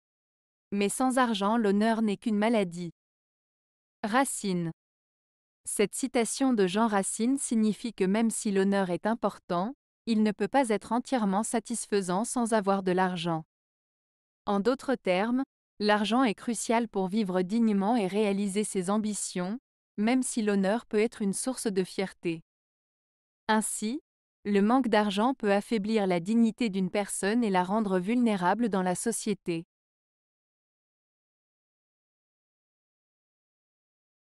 Mais sans argent l'honneur n'est qu'une maladie. - Racine
Cette citation de Jean Racine signifie que même si l'honneur est important, il ne peut pas être entièrement satisfaisant sans avoir de l'argent. En d'autres termes, l'argent est crucial pour vivre dignement et réaliser ses ambitions, même si l'honneur peut être une source de fierté. Ainsi, le manque d'argent peut affaiblir la dignité d'une personne et la rendre vulnérable dans la société.